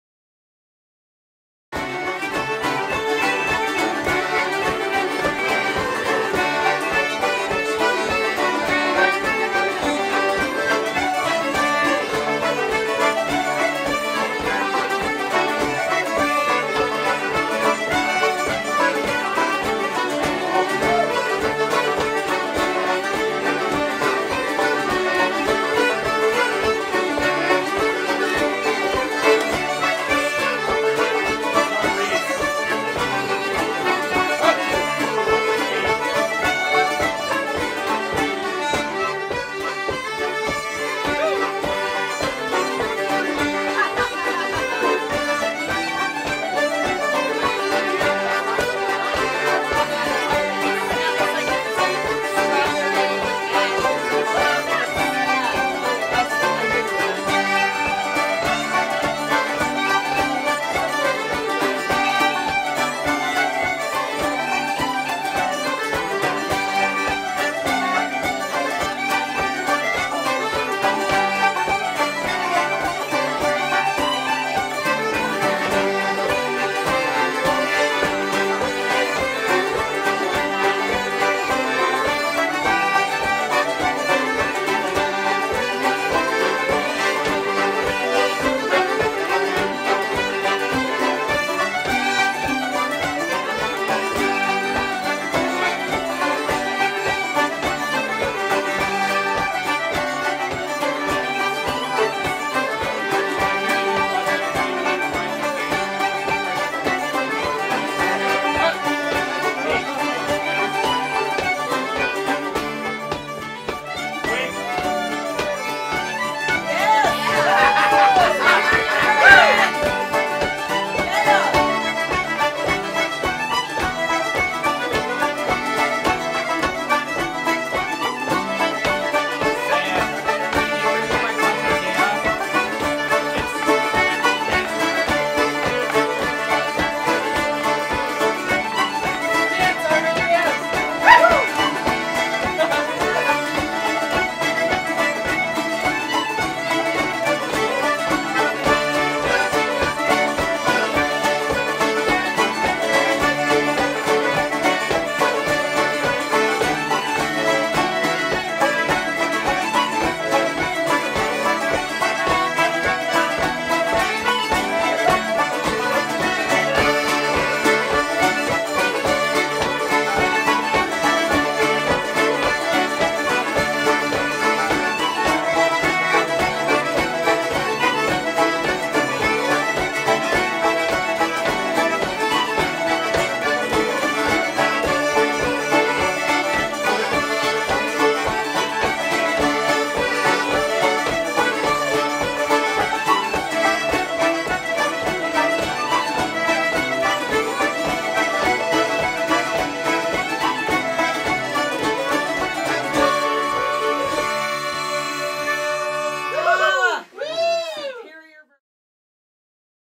Common Room Sessions: Catskills Irish 2015 #74: Friday #5
Old and new friends share tunes and craic on Friday night.  Catskills Irish Week 2015. Tune titles?